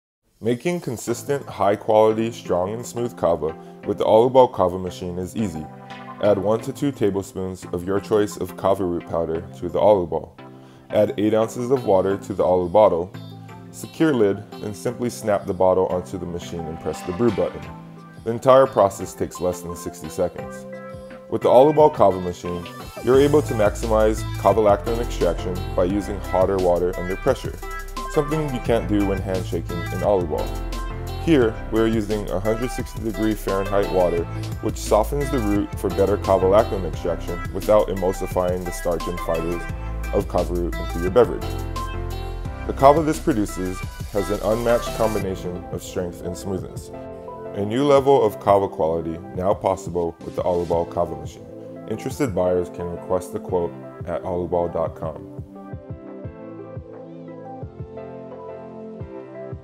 AluBall Kava Brewing Machine - Make the strongest & smoothest Kava with the push of a button
A new level of kava quality now possible with the AluBall Kava Machine 🥥

Making consistent high quality strong and smooth kava with the aluball kava machine is easy. Add 1-2 tbsp. of your choice of kava root powder to the aluball.

Add 8 ounces of water to the alubottle. Secure lid then simply snap bottle onto the machine and press the brew button. The entire process takes less than 60 seconds.

With the AluBall kava machine you are able to maximize kavalactone extraction by using hotter water under pressure, something you can’t do when hand shaking an aluball. Here we are using 160F degree water. This softens the root for better kavalactone extraction without emulsifying the starch and fibers of kava root into your beverage.

The kava this produces has an unmatched combination of strength and smoothness; A new level of kava quality now possible with the Aluball Kava Machine.